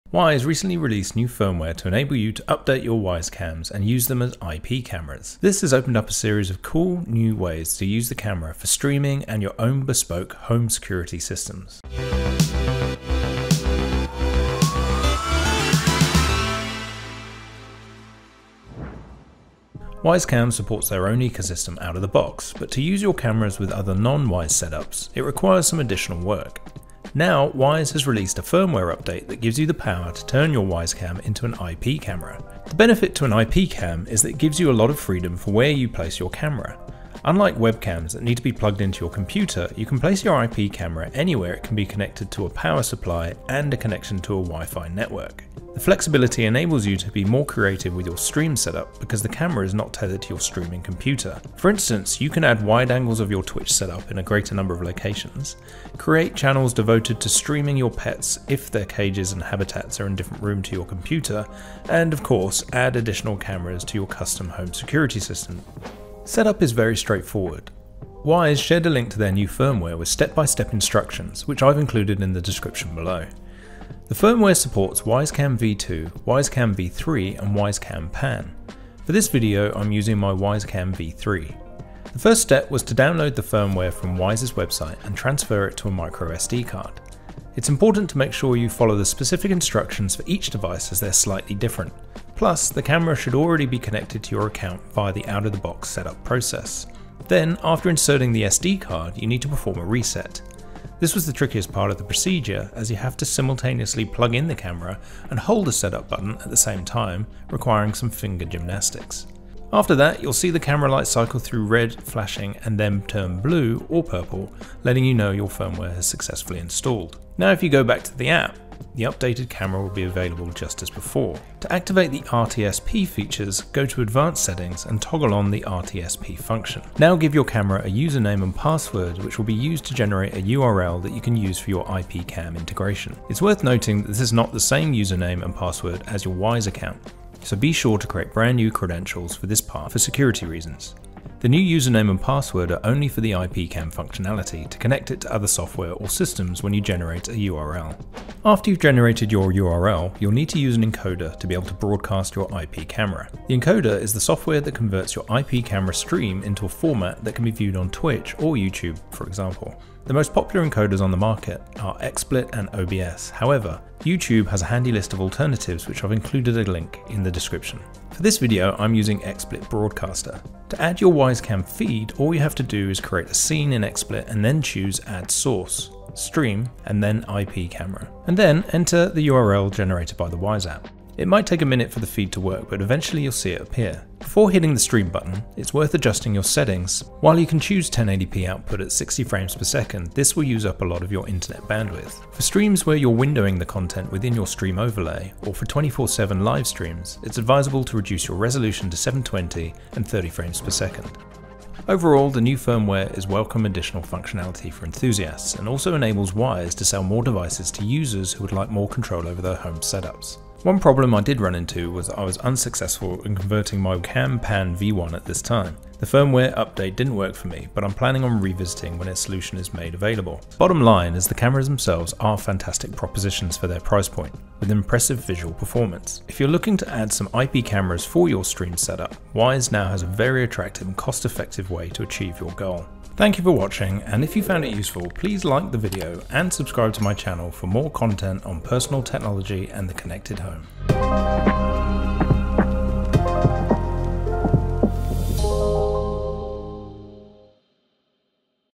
How to Turn your $35 Wyze Cam into an HD IP Camera for Streaming!
Wyze recently released new firmware to enable you to update your Wyze Cams and use them as IP cameras.

This has opened up a series of new cool ways to use the camera for Twitch and YouTube streaming and your own bespoke home security systems.

00:00 - Intro
01:05 - Setup
02:28 - Streaming
03:21 - Summary